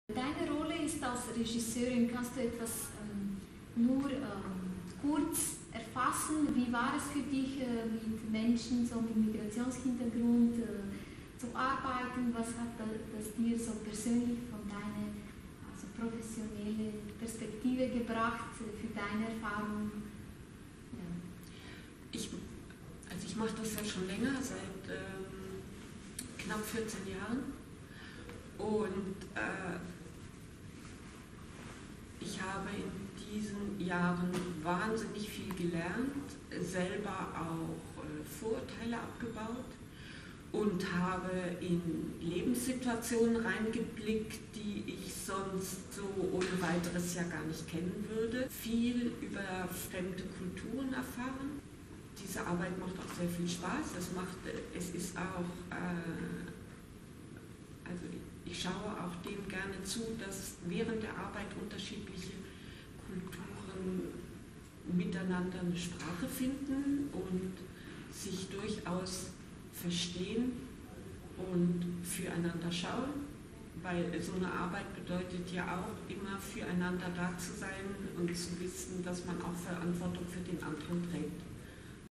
Mit Schauspieler*innen mit Migrationshintergrund arbeiten?
Im Gespräch mit Jasmine Hoch, die Regisseurin vom Theaterstück "I am Walking on Sunshine" in der Produktion vom MAXIM Theater Zürich